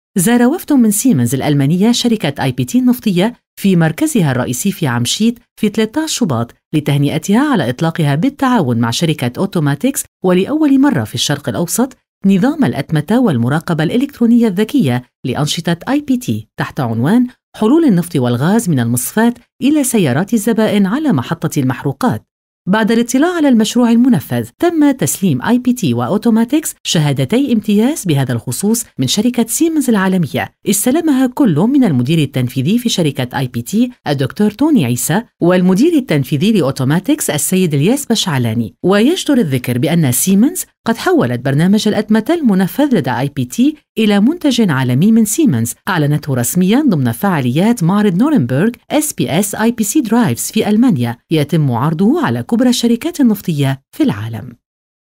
IPT's Automated Fuel Monitoring System: Launched in Lebanon & Spread Worldwide- VNR 60''
On the 13th of February 2017, representatives from Siemens International visited IPT headquarters in Amchit to congratulate IPT Group and AutomatiX for Launching "the First Entire Supply Chain Automated Fuel Monitoring System in the Middle East" under the title of “Oil and Gas solutions, from the refinery to the customers’ vehicles on Gas stations”.

After checking the project, Siemens International presented certificates of excellence for IPT, represented by Dr. Toni Issa, president of IPT Energy Center (IPTEC), and AutomatiX, represented by Mr. Elias Bechaalani, CEO.

Siemens transformed this automated system into a global product announced in its Exhibition in Germany, SPS IPC Drive.

The purpose of this system is to offer complete remote and accurate management and control of all the operations on the stations, covering the entire supply chain from the unloading of petroleum products at IPT terminal, to the storage, distribution and sales on gas stations.

This monitoring system will allow a full control of the quality and quantity of our products from the tanks to the consumers.